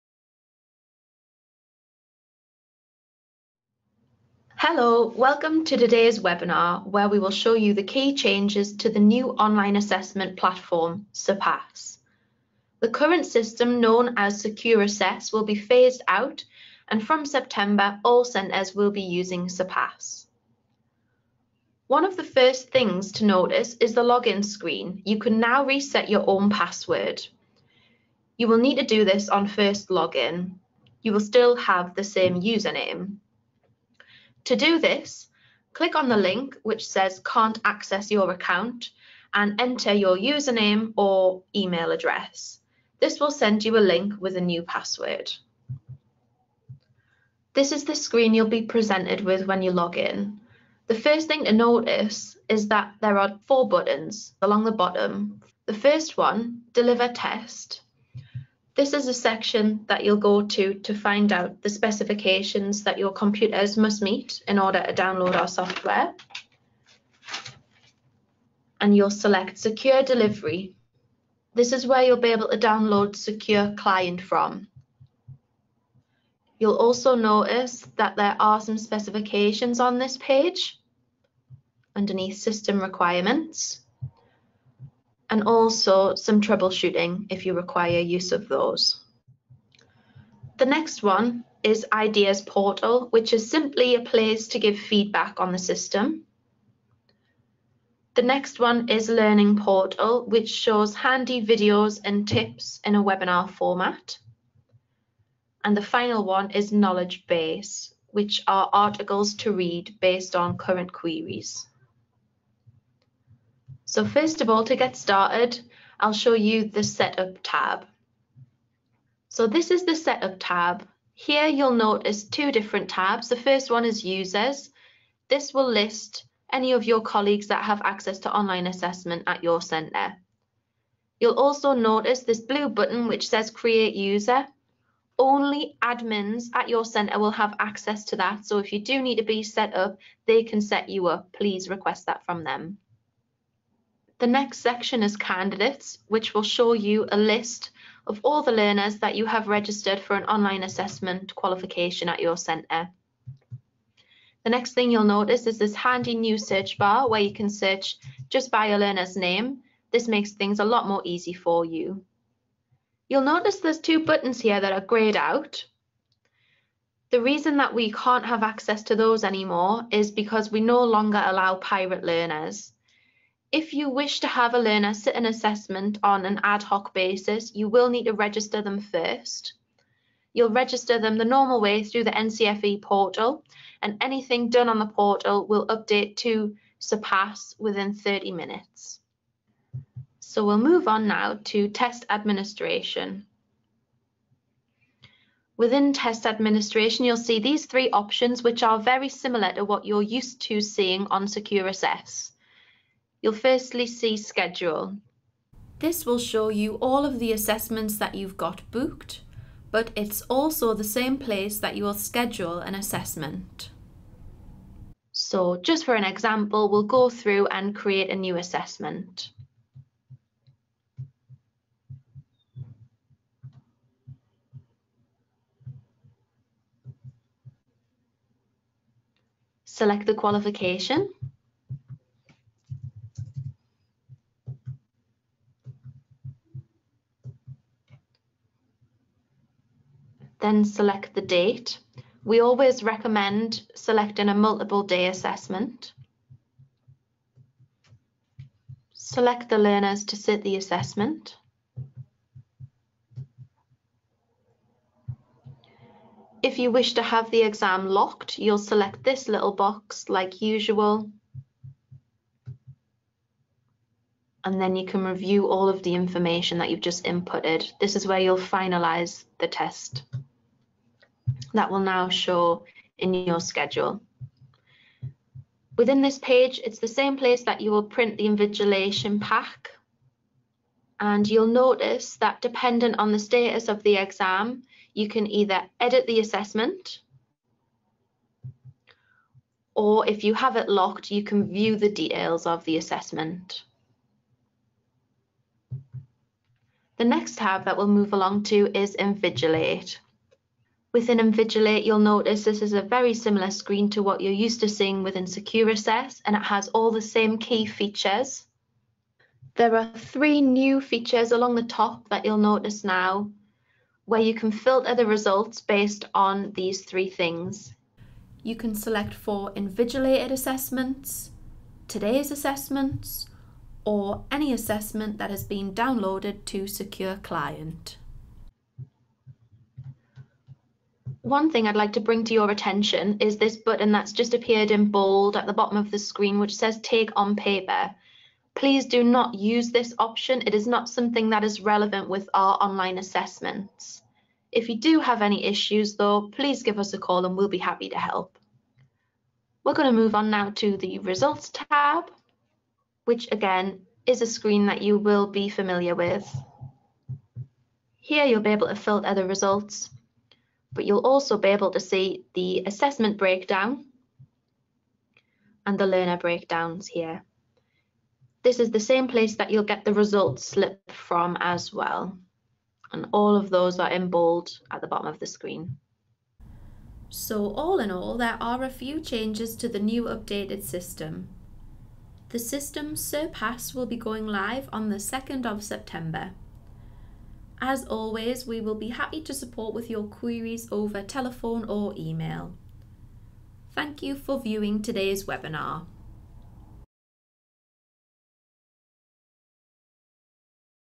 Surpass Online Assessment Webinar
Join us as we walk you through the key changes to our new Online Assessment platform Surpass.